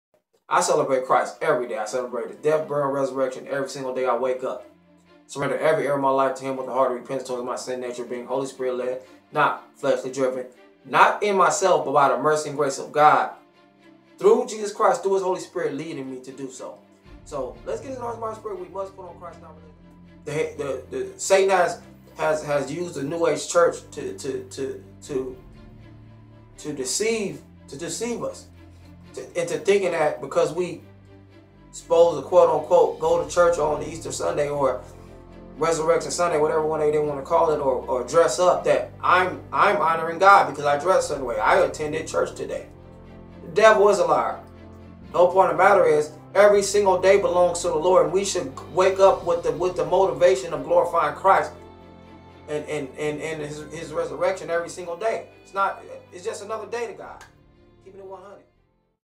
THE GOSPEL MOVEMENT YO! Pastor Rodney Hewitt "We must put on Christ not religion!" pt 3
Romans 13:12-14 Colossians 2:6-10,16-19 St. John 4:23-24 1Samuel 16:7😄😄💪💪🔥🔥😍😍